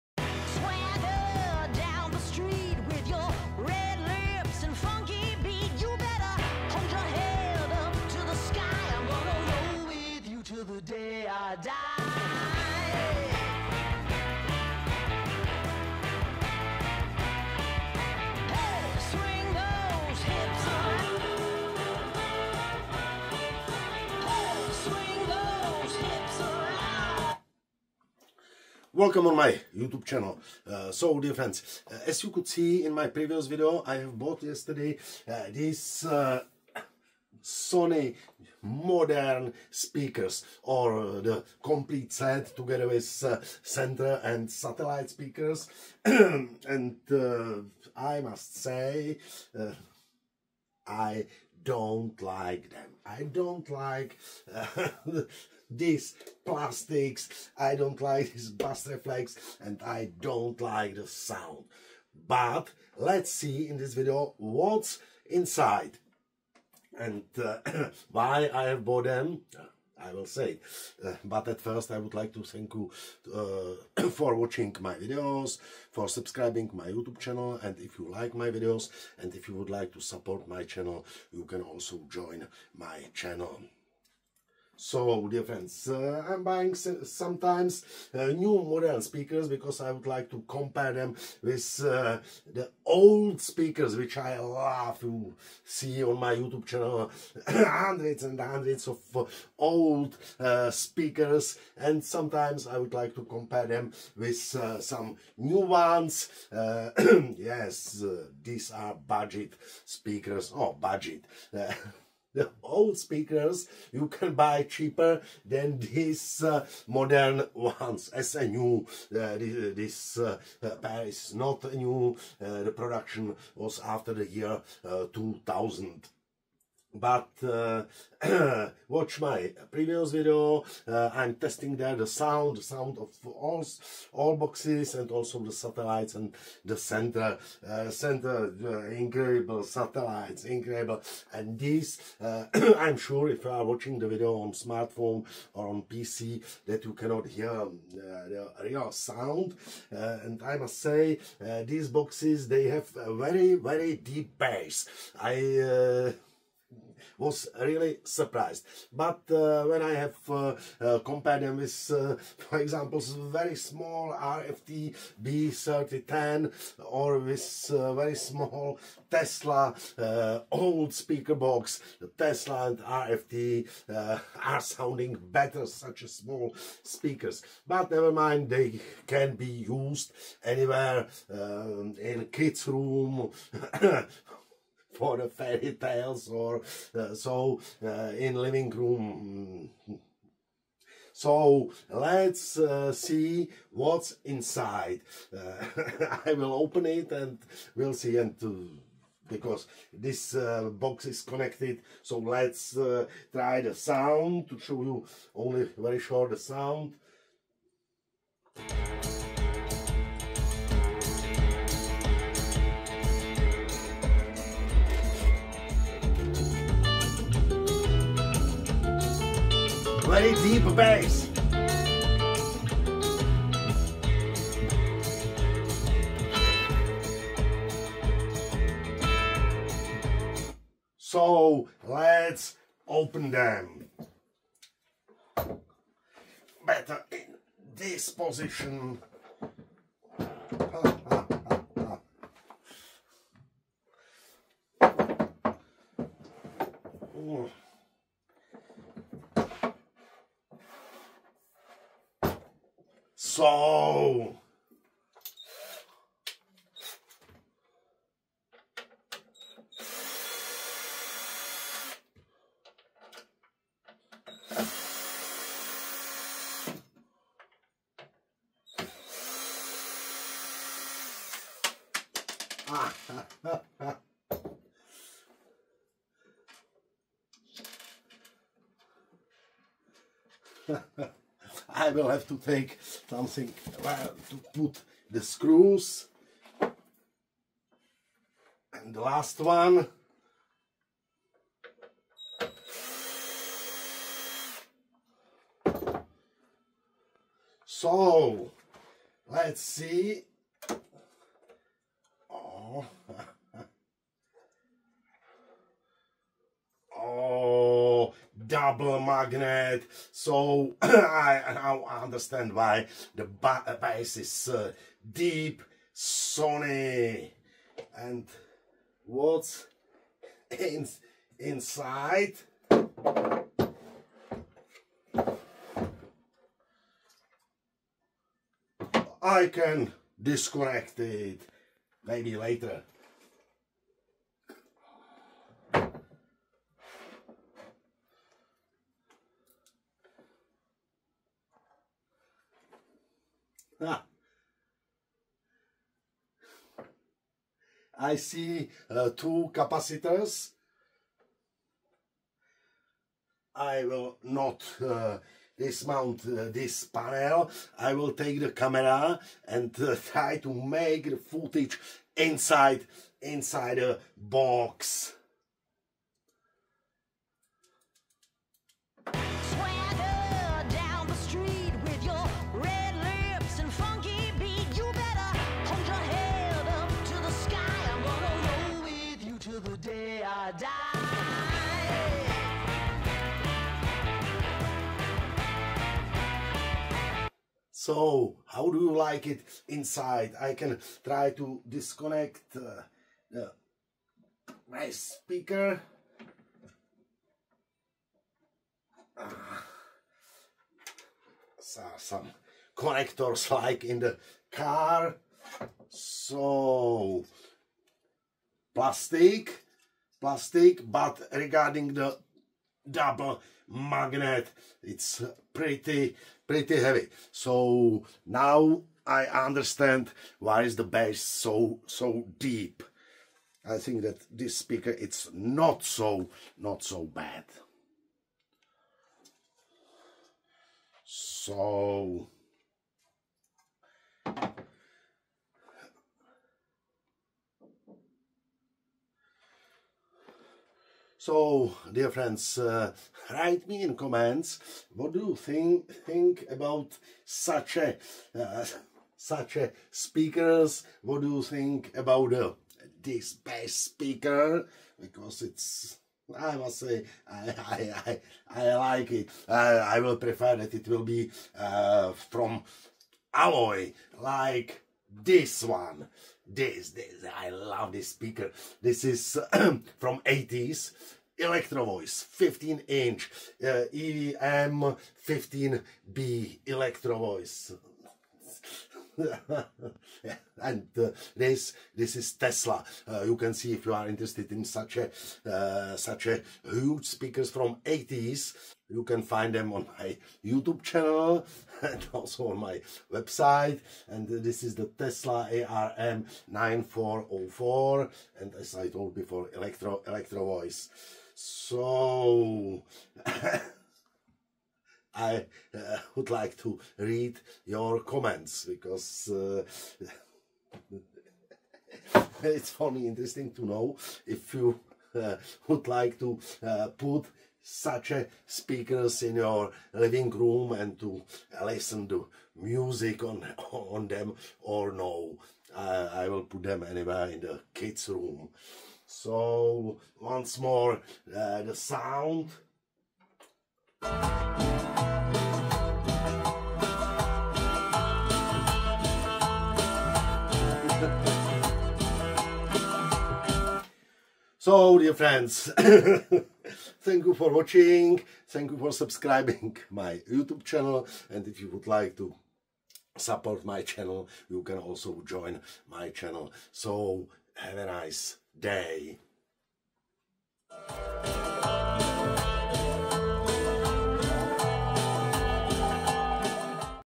Budget Speaker Dissection: Sony SS-MF450H Value
Look inside - Double Magnet BASS Speaker - Teardown: The inner workings of a budget-friendly speaker are carefully unveiled, offering an insightful look into its construction and surprising value. A detailed dissection is performed, demonstrating how unexpected quality can be found within an accessible price point. What if I told you the secret to incredible audio doesn't have to break the bank? We're about to crack open a budget speaker, the Sony SS-MF450H, and expose the surprising truth about its internal design. Prepare to be amazed by the unexpected quality hiding inside this affordable sound system. Can a speaker that costs less than dinner truly deliver an amazing audio experience? Most people would say no, but I'm here to challenge that assumption. Join me as we dissect the Sony SS-MF450H and uncover the engineering choices that make this budget speaker a hidden gem of value. @Angelicaaudio @youtubecreators